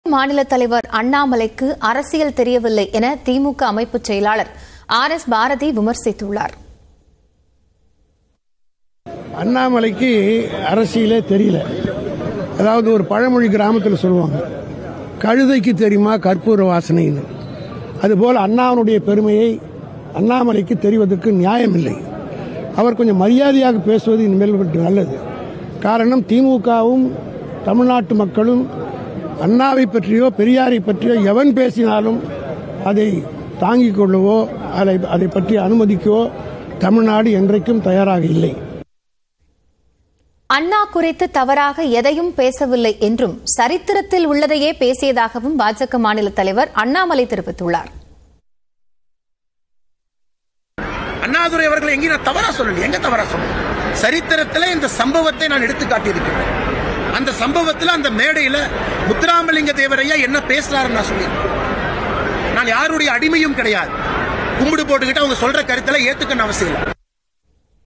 DMK vs BJP | கழுதைக்கு தெரியுமா கற்பூர வாசனை.. அண்ணாமலையை விமர்சித்த ஆர்.எஸ். பாரதி | Annamalai
அண்ணா மற்றும் பெரியார் குறித்து அவதூறாக பேசுவதை பாஜக மாநில தலைவர் அண்ணாமலை நிறுத்திக்கொள்ள வேண்டும்.  தமிழ்நாடு மக்கள் வெகுண்டு எழுவார்கள் என திமுக அமைப்பு செயலாளர் ஆர்.எஸ்.பாரதி எச்சரிக்கை.